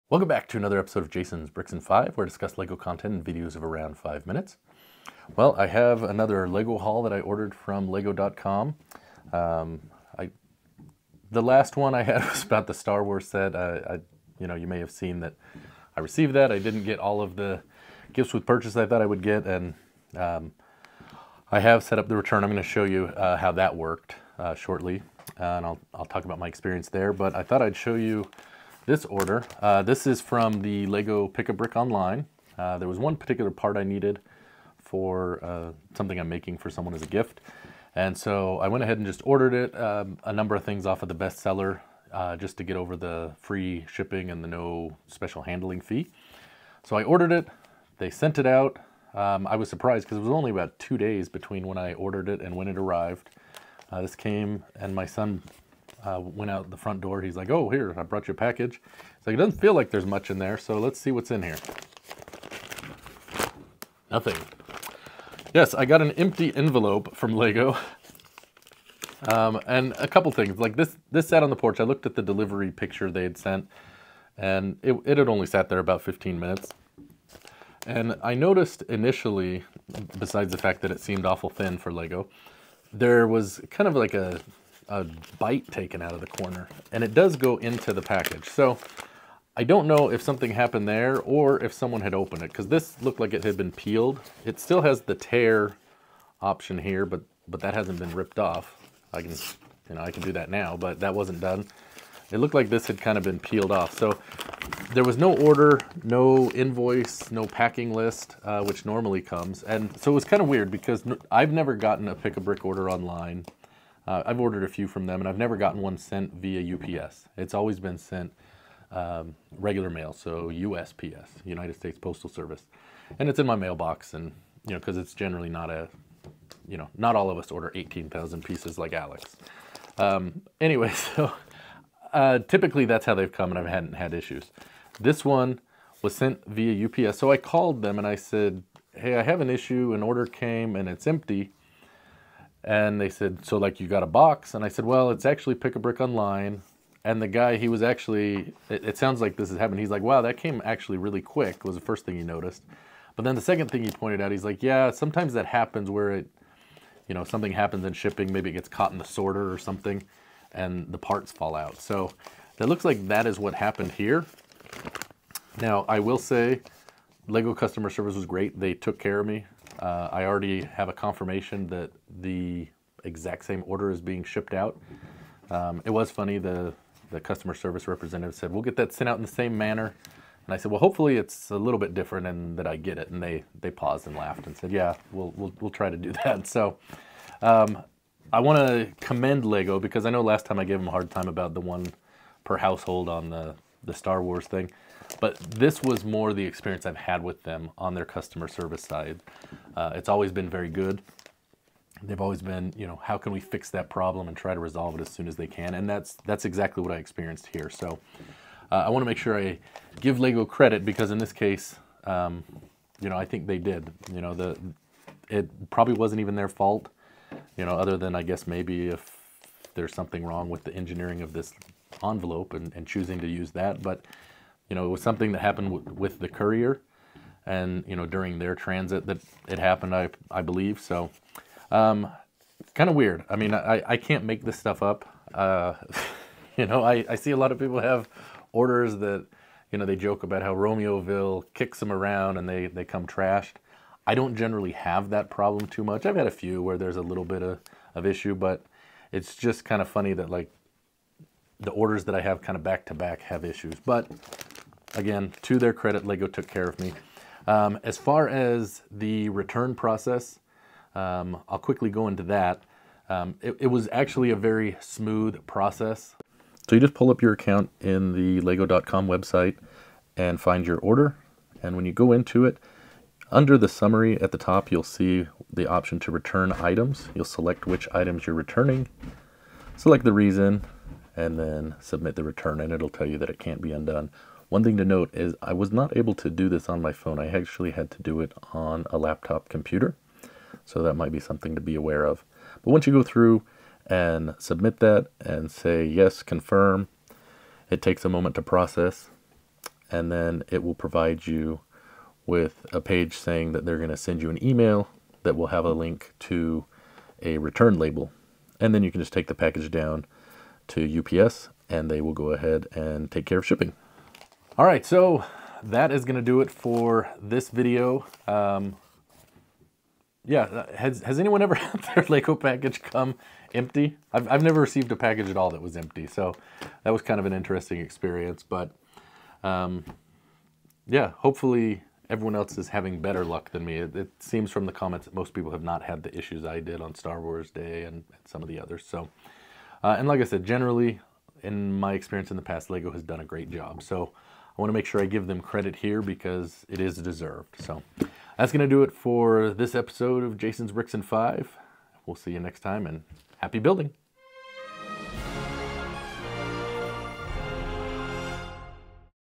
I received an empty Lego package? | Plus how easy is it to process a return on Lego.com?
I received a Lego.com pick a brick order and the package was empty! It’s like my buddy @Mardigrasman23 said “you can’t make this stuff up”!